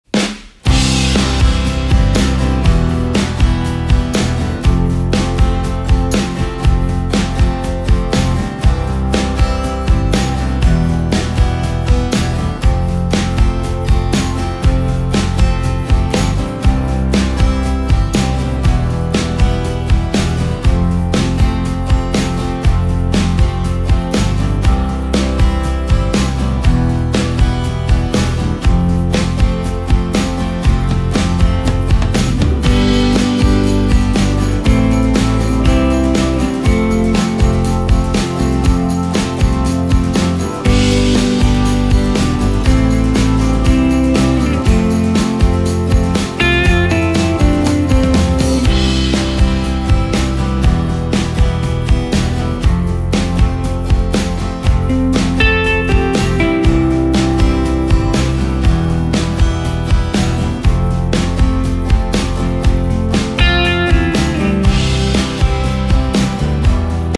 Brother ADS-4300N Network Desktop Document Scanner.
Brother ADS-4300N Professional Desktop Scanner with Fast Scan Speeds, Duplex, and Networking.
Media Type Paper
Scanner Type Document
Brand Brother
Model Name ADS-4300N
Connectivity Technology USB, Ethernet